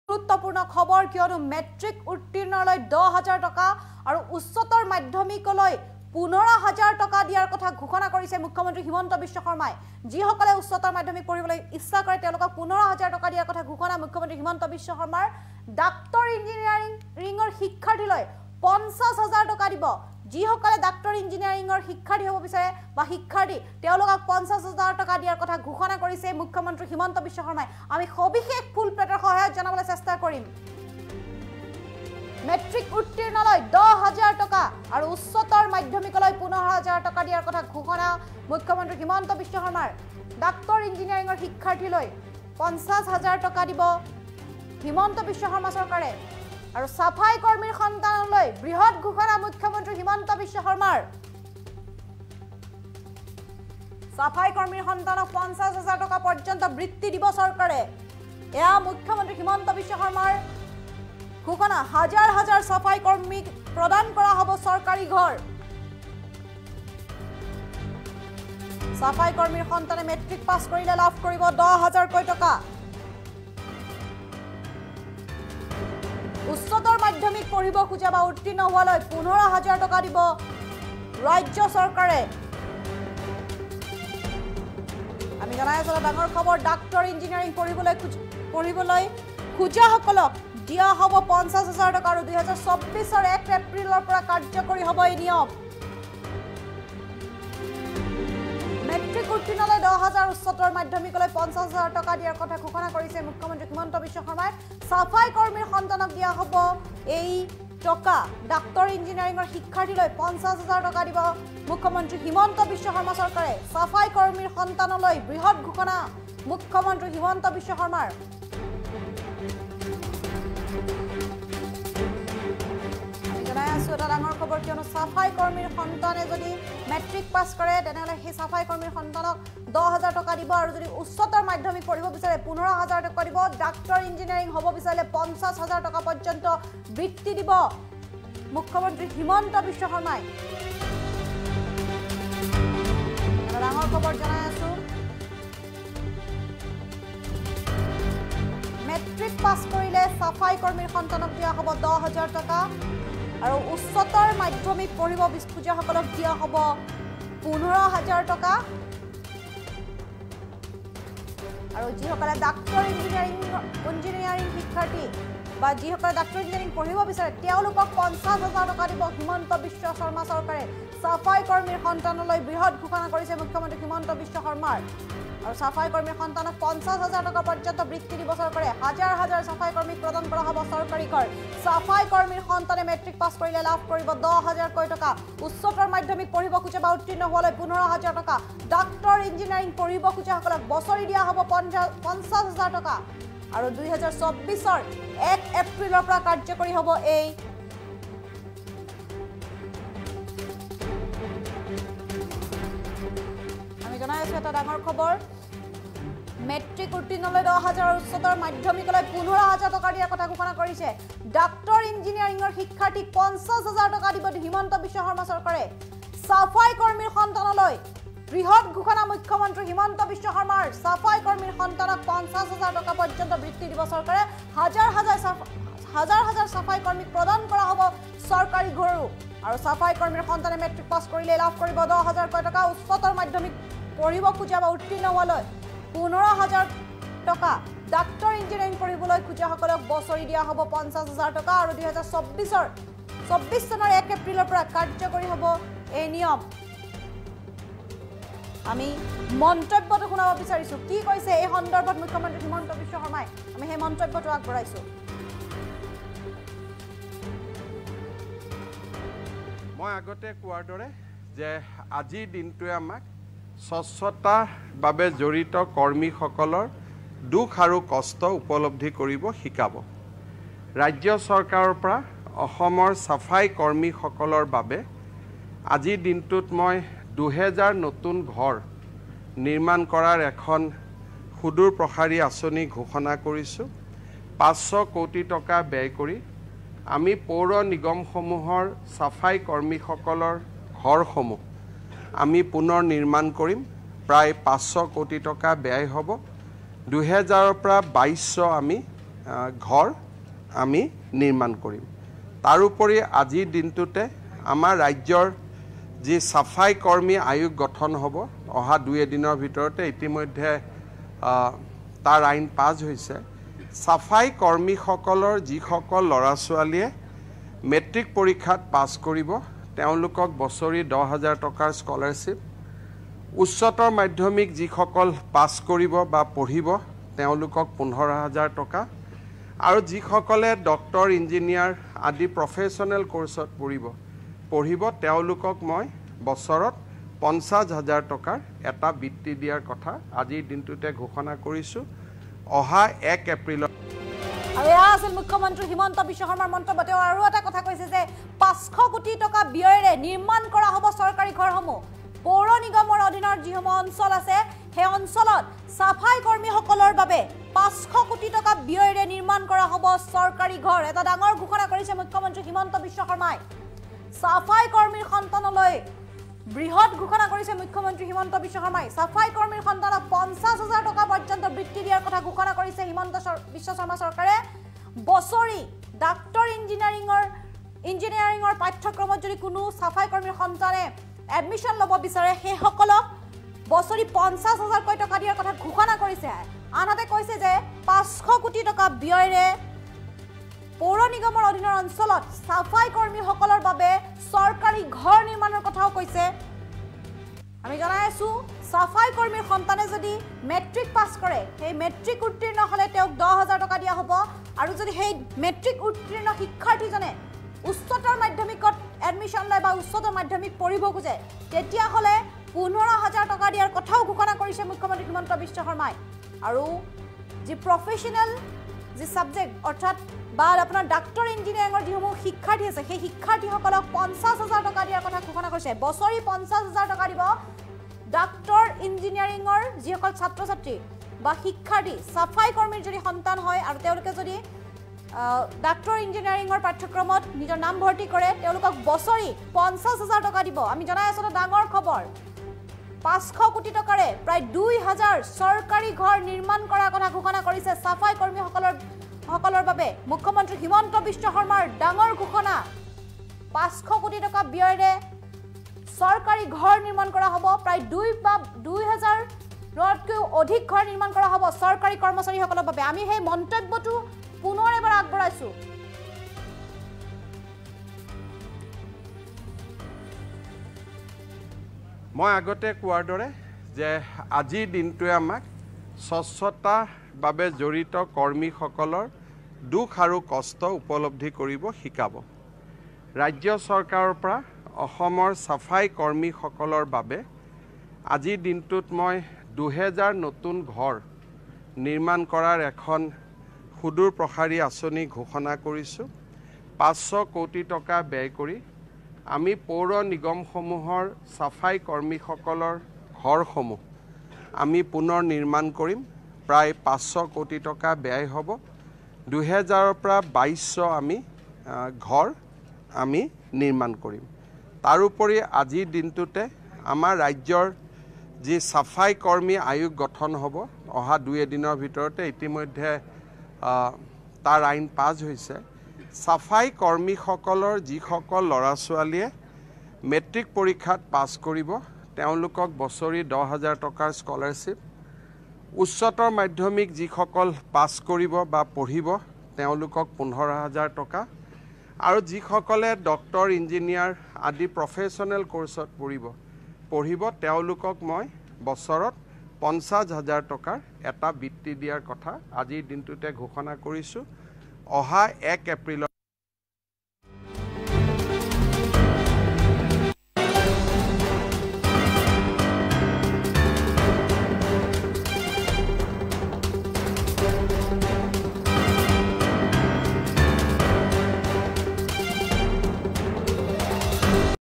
মেট্ৰিক উত্তীৰ্ণলৈ ১০ হাজাৰ, উচ্চতৰ মাধ্যমিকলৈ ১৫ হাজাৰ টকা দিয়াৰ ঘোষণা
মেট্ৰিক উত্তীৰ্ণলৈ ১০ হাজাৰ, উচ্চতৰ মাধ্যমিকলৈ ১৫ হাজাৰ টকা দিয়াৰ ঘোষণা। ডাক্টৰ, ইঞ্জিনিয়াৰিঙৰ শিক্ষাৰ্থীলৈ ৫০ হাজাৰ টকা দিব হিমন্ত বিশ্ব শৰ্মা চৰকাৰে। চাফাই কৰ্মীৰ সন্তানলৈ ঘোষণা মুখ্যমন্ত্ৰীৰ